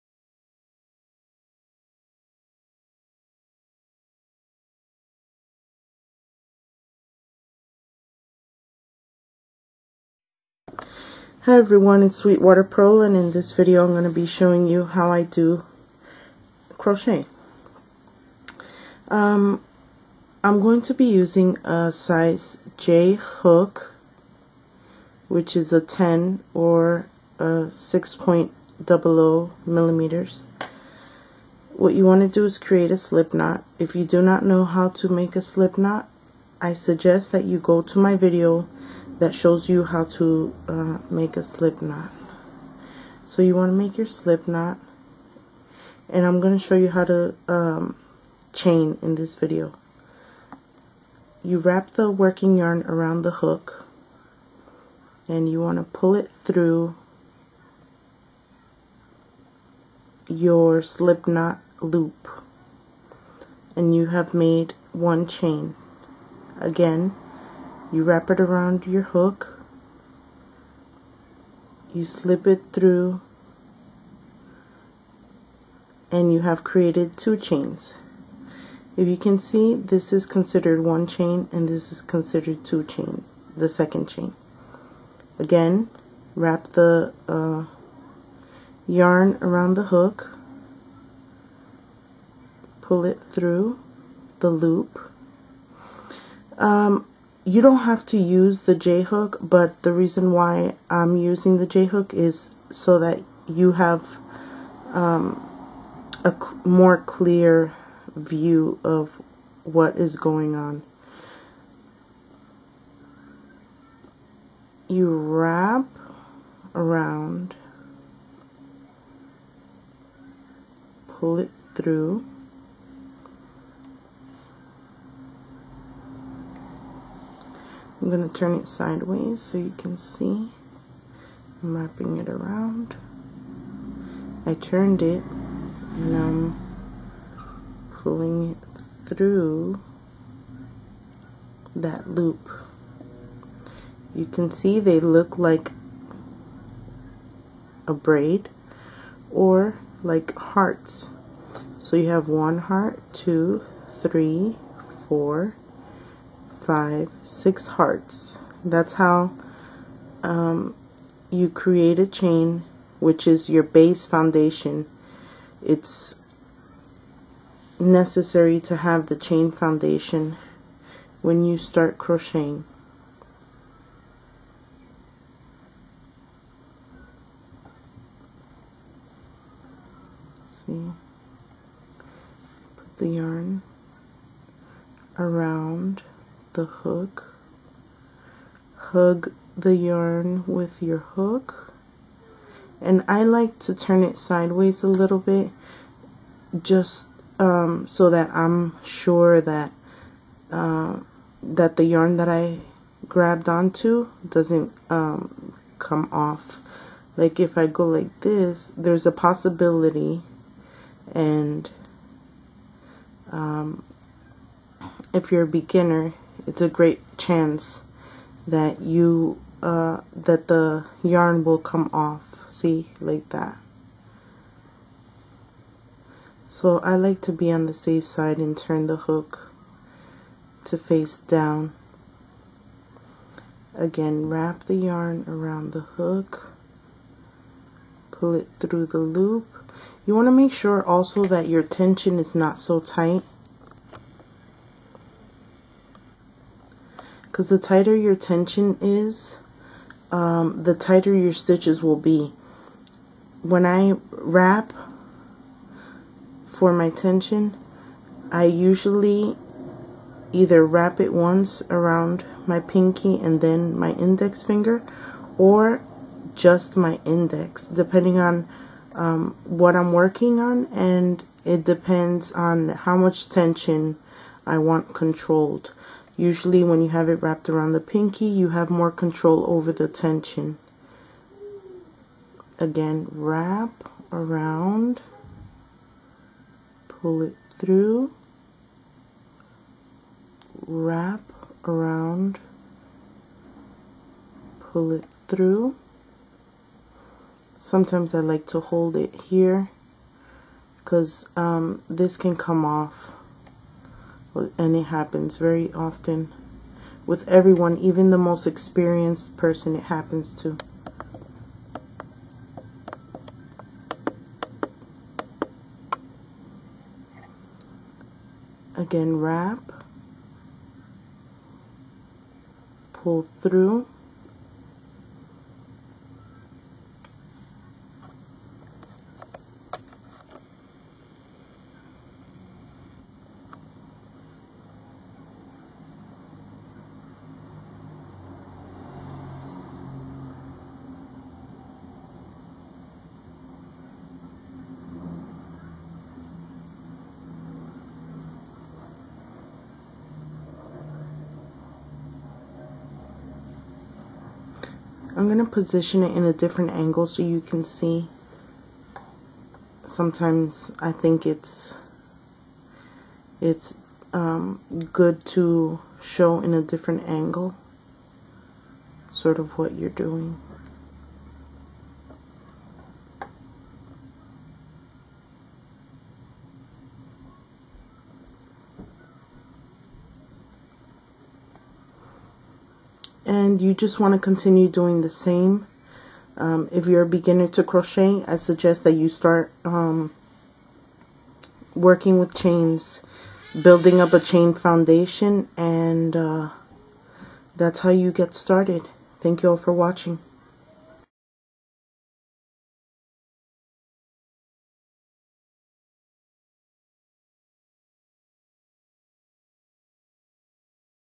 Crochet: Chain Foundation
In this video you will learn How to create a Chain Foundation when crocheting... Highly Detailed Video